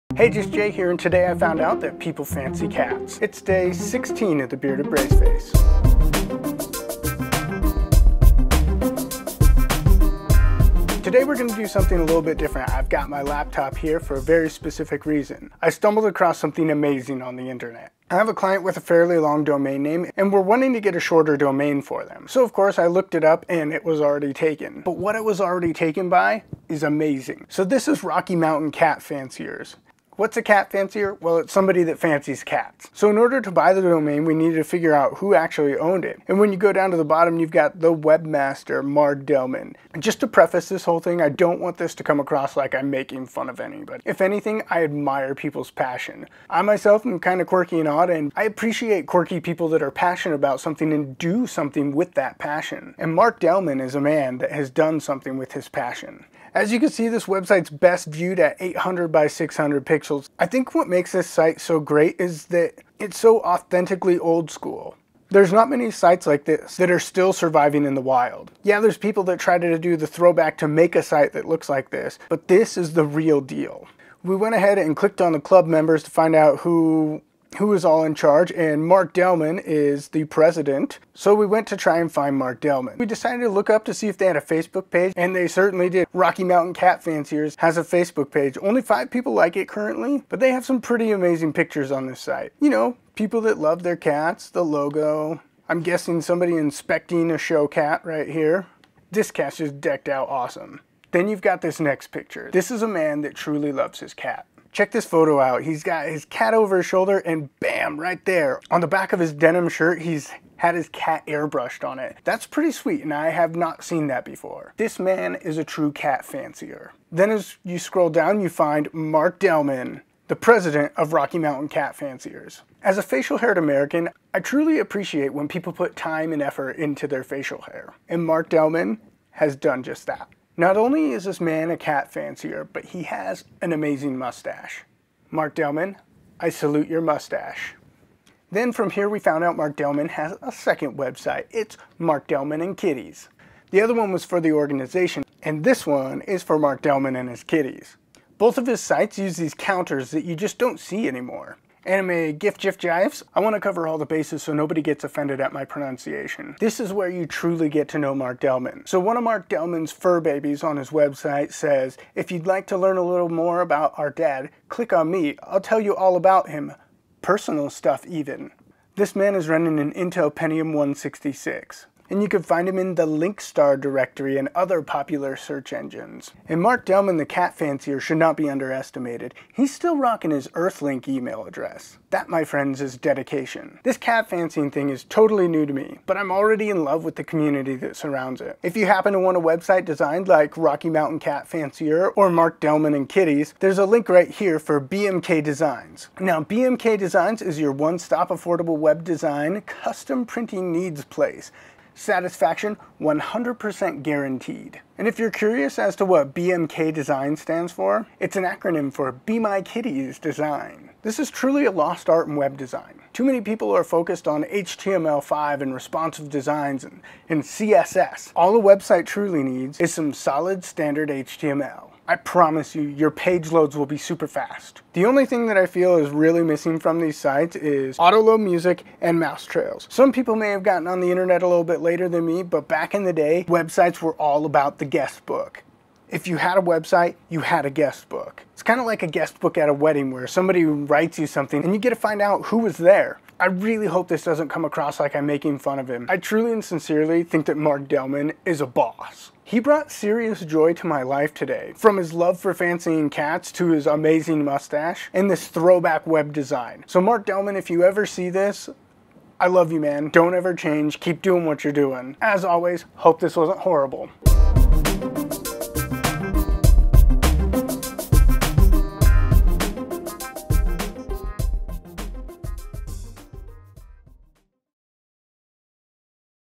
Day 016 - Cat Fancier: Mark Delman
This is my journey. A man in my 30’s navigating the life of being bearded and having adult braces.

Today I stumbled on Cat Fanciers. It was so interesting to me to start skimming the surface of this hobby, interest, profession, whatever you want to classify it as. And just like anything I think it's AWESOME people have found something they are passionate about and are able to find other like-minded people. Like Bronies, Cosplayers, Sports Fanatics, Filmmakers, Crafters, Singers, Dancers, whatever, it's just cool to see. Mark Delman just happens to be the guy that introduced me to this and has also kept some of the old school internet alive (which I totally appreciate... I recently tried to find some of my old sites... which sadly have pretty much all disappeared. Angelfire, geocites, and whatever other free 150MB hosting sites used to be out there).